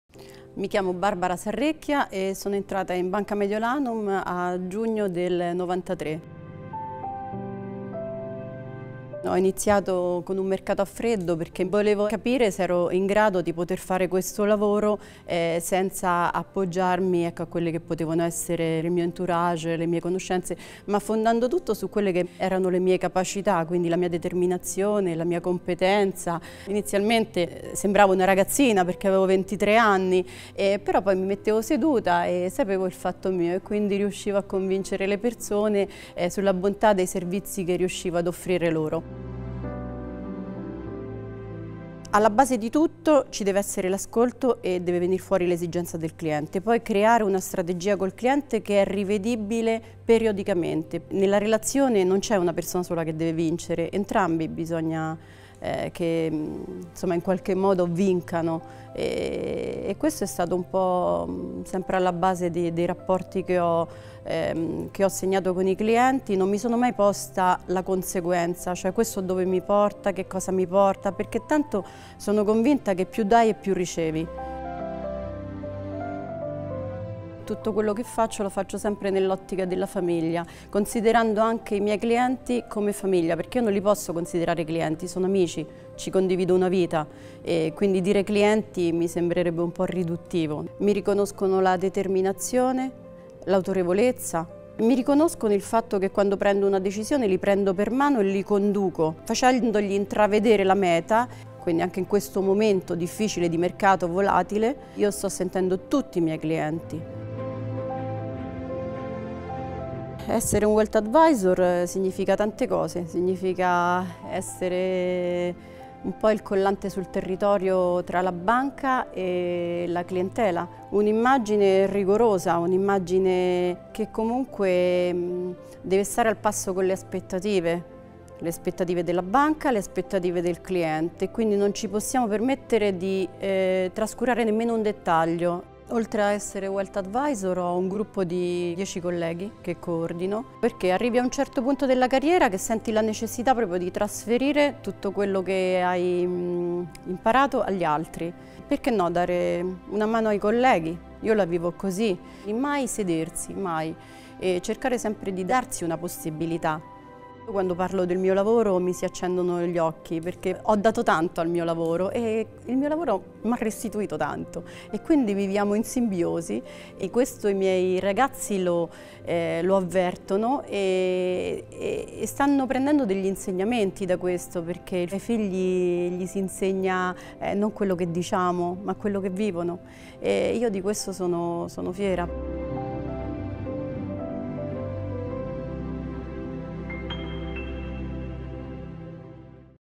Wealth Advisor | Barbara Sarrecchia
I Wealth Advisor di Banca Mediolanum offrono, oltre ad una consulenza personalizzata in ambito finanziario, supporto e assistenza nella scelta delle migliori soluzioni finalizzate alla valorizzazione del patrimonio della clientela e alla conservazione nel tempo del tenore di vita, anche in un'ottica di pianificazione successoria e di ottimizzazione fiscale.